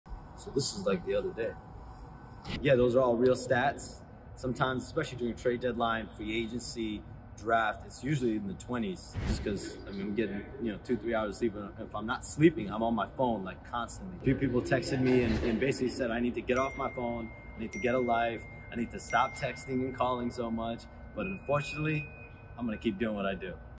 Shams Charania Phone Screen Time Numbers Are Crazy 🤯
It might not come to anyone's surprise but Shams Charania has some insane numbers on his screen time. The NBA insider recently showed his screen time numbers and he spends a lot of time on his phone reporting the big NBA news.